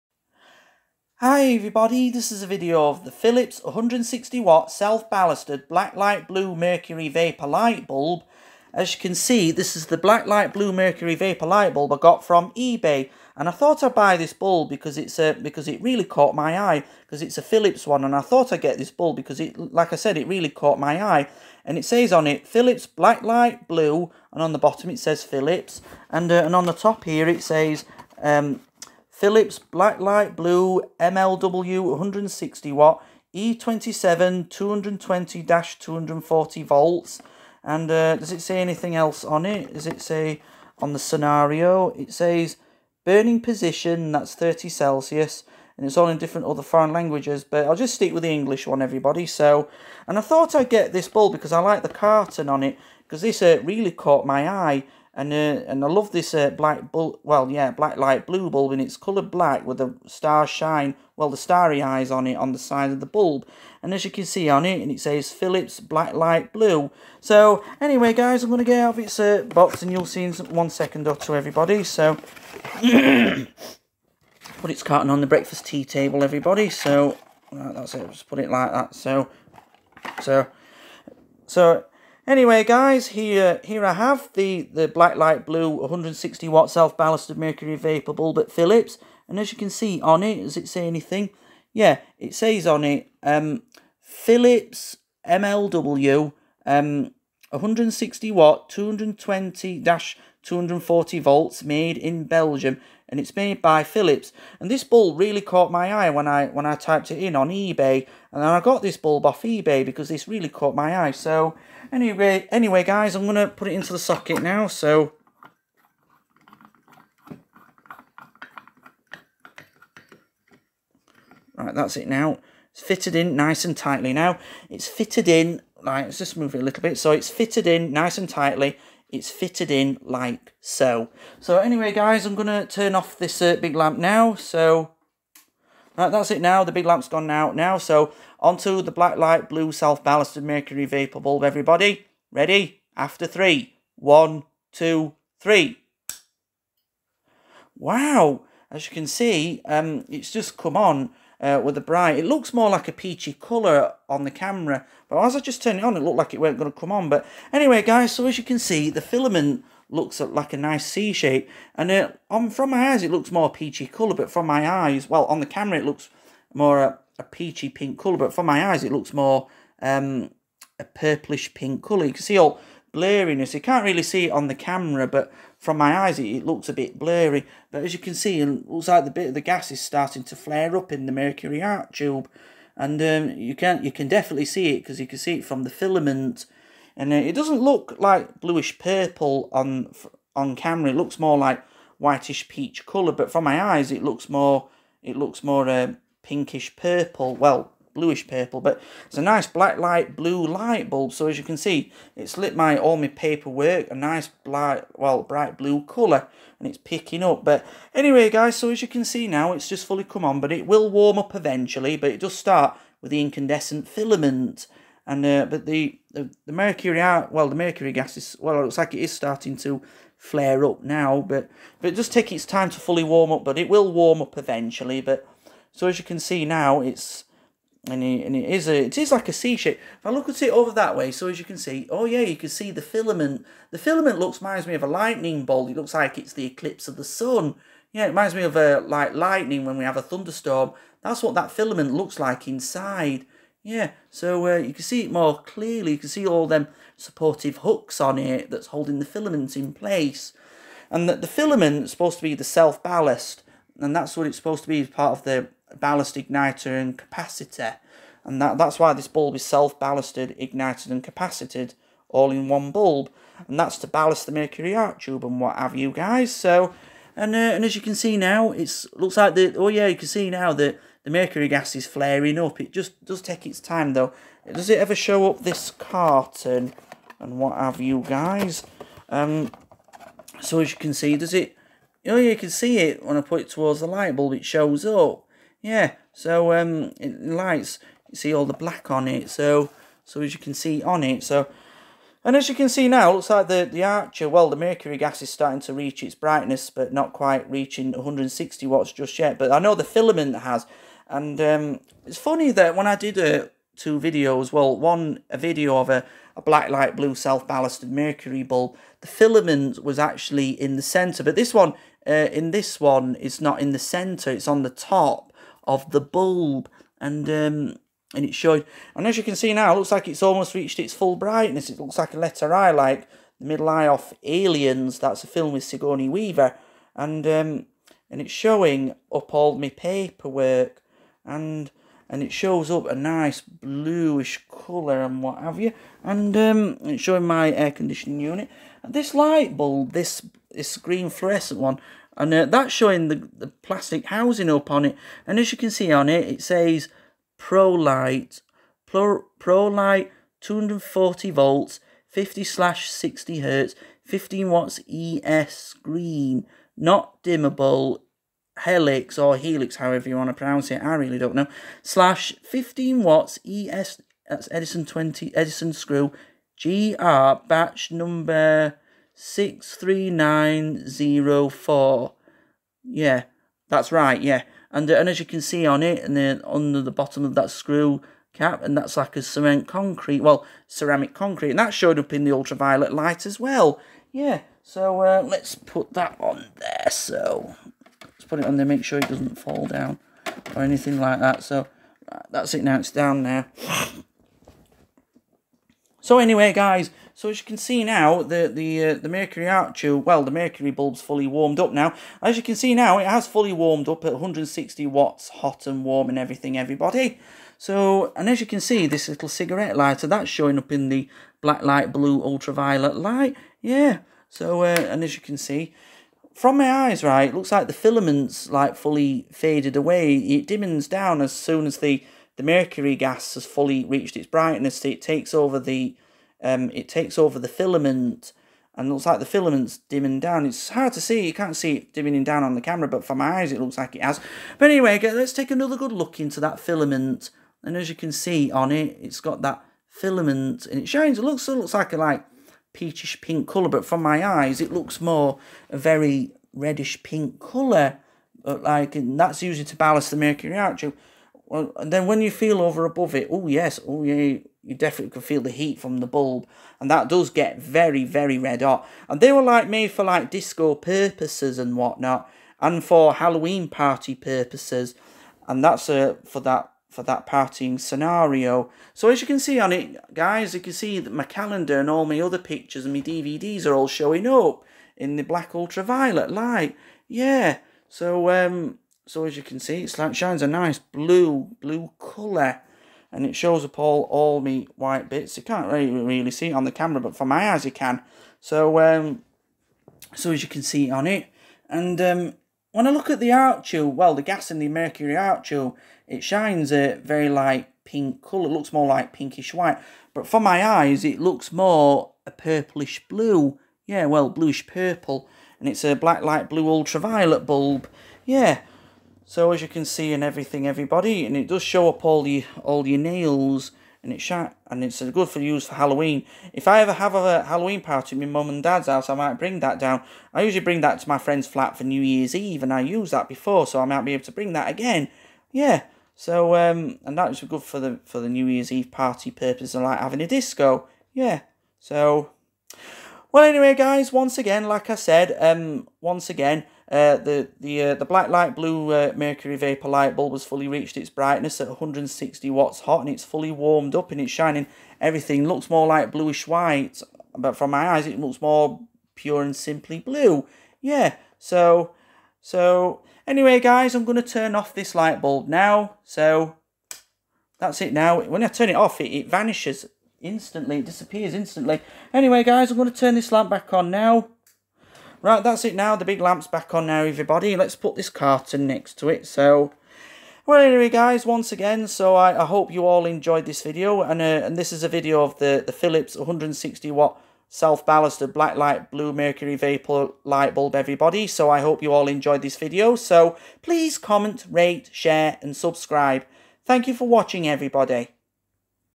Philips 160 watt self ballasted blacklight blue mercury vapor lightbulb
This mercury vapor bulb 💡was bought off Ebay as it really caught my eye, the filament is at the top and the arc tube is halfway in the centre and that's how Philips made the bulb, it's blacklight blue at 160 watts.